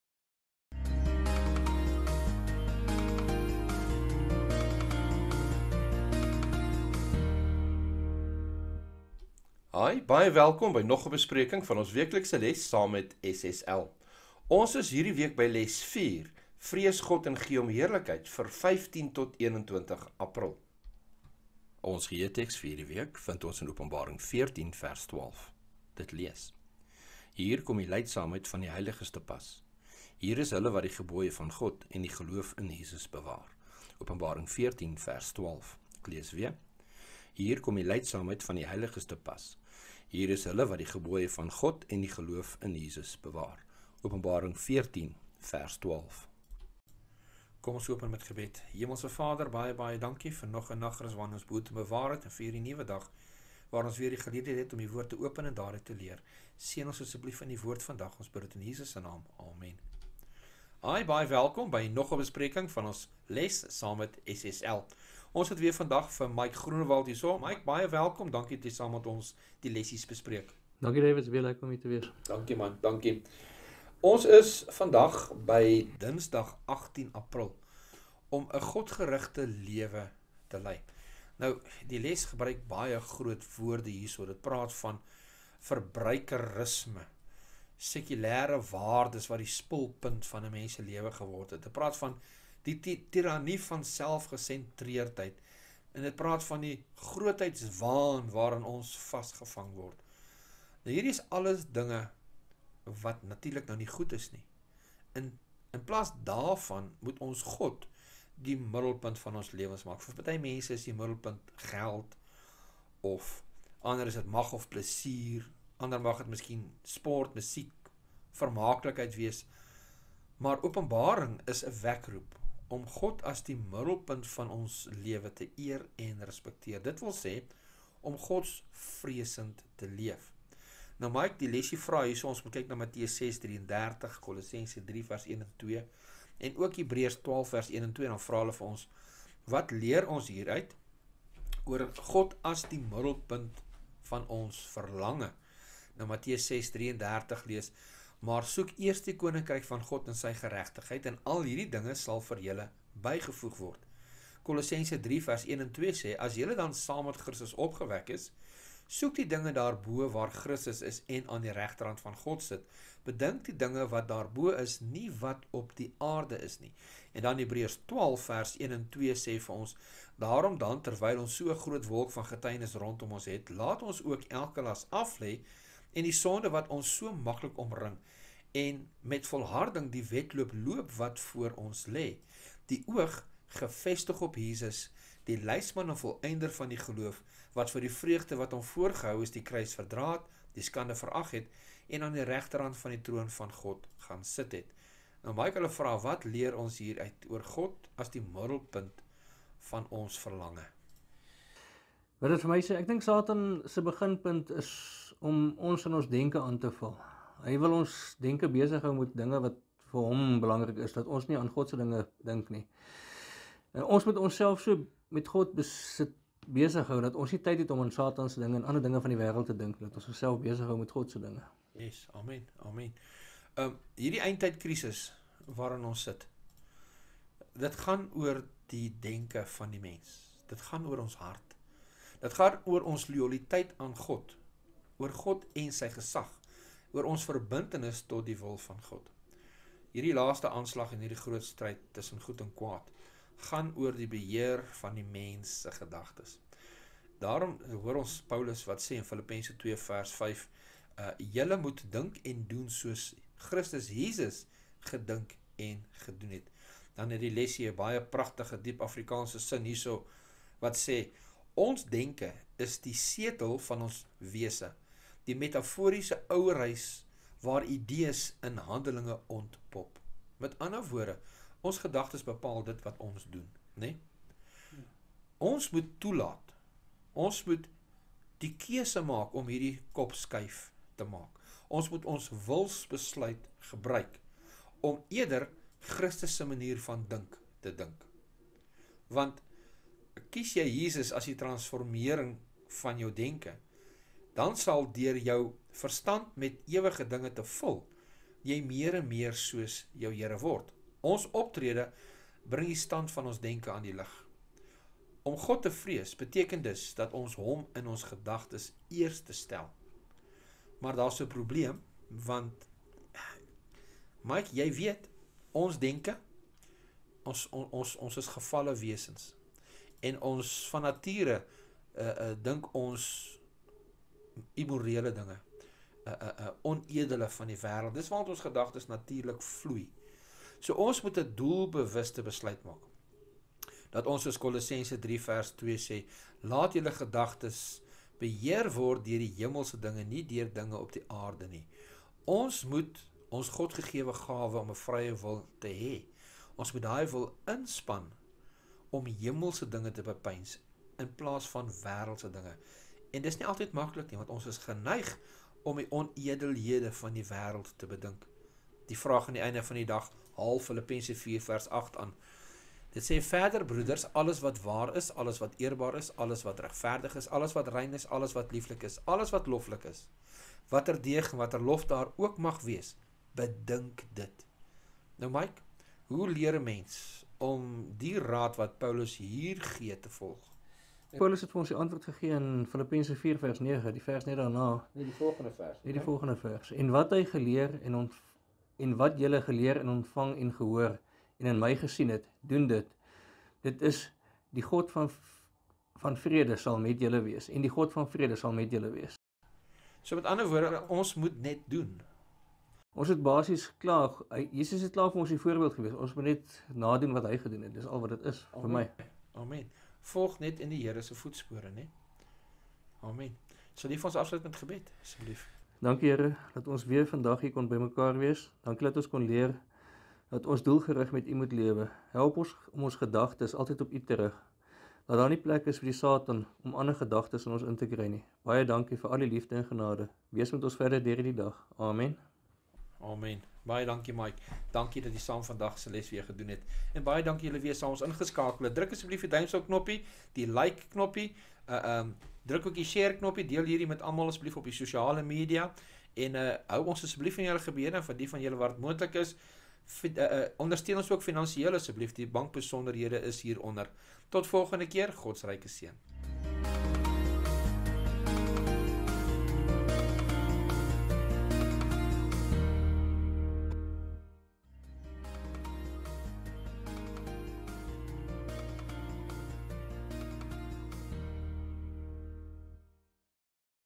Dinsdag 18 April - Om ’n Godgerigte lewe te lei.
Les 4 – Vrees God en gee hom heerlikheid
15 tot 21 April
Twede Kwartaal 2023 – Drie Kosmiese Boodskappe

Aanbieder:
David Kühn
Gas-spreker:
Mike Groenwald

Jesus Christus.